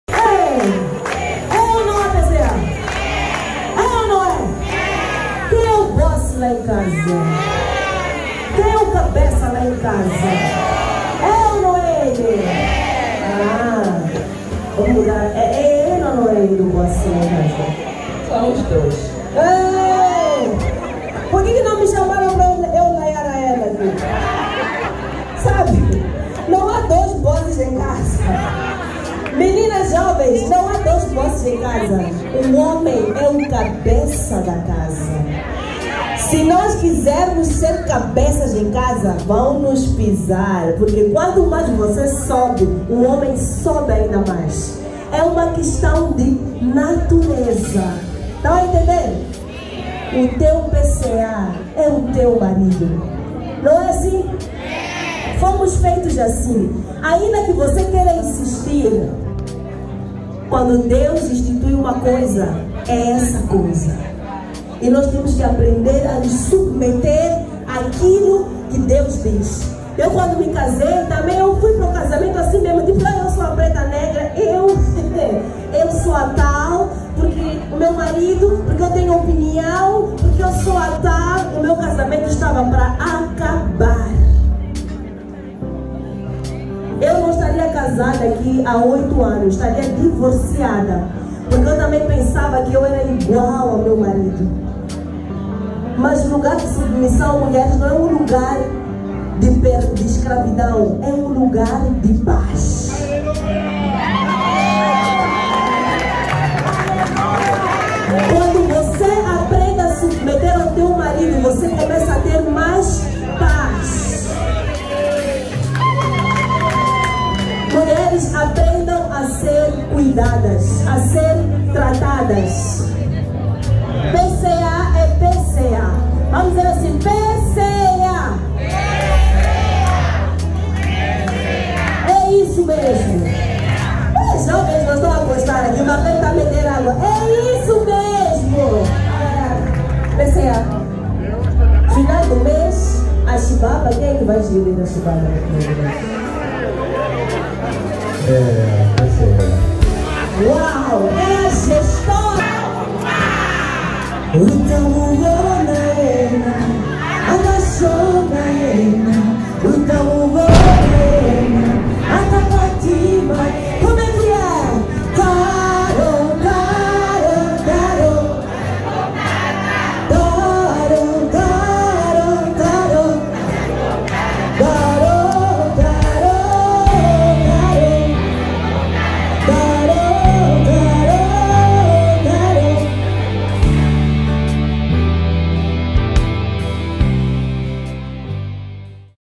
Marllen fala sobre a submissão da mulher ao marido no amor de Deus
"As mulheres sejam submissas ao seu próprio marido, como ao Senhor; porque o marido é o cabeça da mulher, como também Cristo é o cabeça da igreja, sendo este mesmo o salvador do corpo" (Efésios 5:22-23)
Queridas, tudo o que DEUS estabeleceu é bom, agradável e perfeito. Se seguirmos com fé estaremos sempre na posição de segurança e de vitória